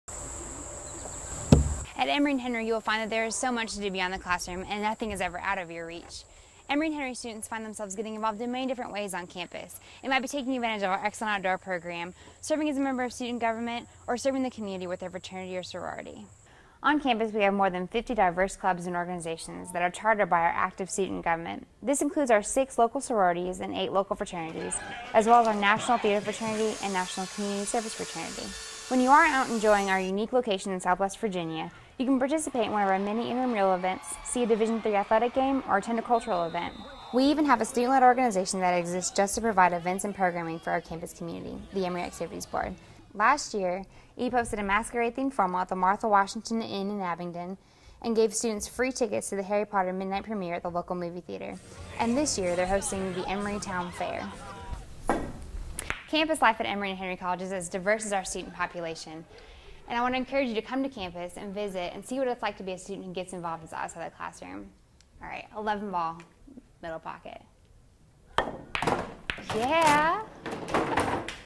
Alex Veatch - Campus Life @ Emory & Henry
Admission's counselor Alex Veatch gives us a glimpse at what life on the Emory & Henry campus has to offer.